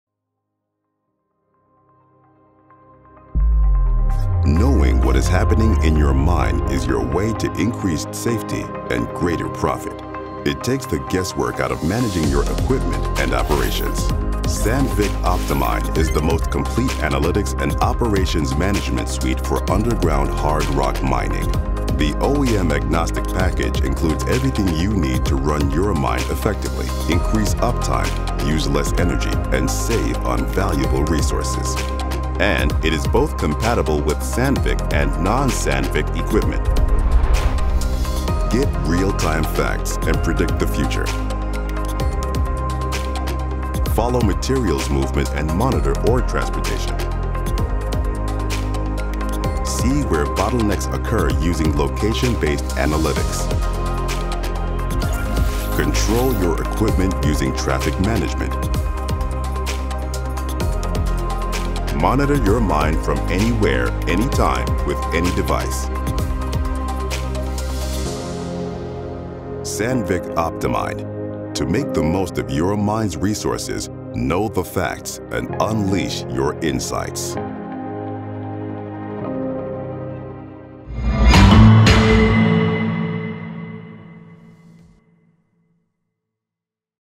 OptiMine® analytics and process optimization | Sandvik Mining and Rock Technology
OptiMine® is the most comprehensive suite of digital solutions for analyzing and optimizing mining production and processes underground. It integrates with Newtrax IoT devices, delivering data from all assets, people and equipment – including non-Sandvik equipment – into one source, providing real-time and predictive insights to improve operations.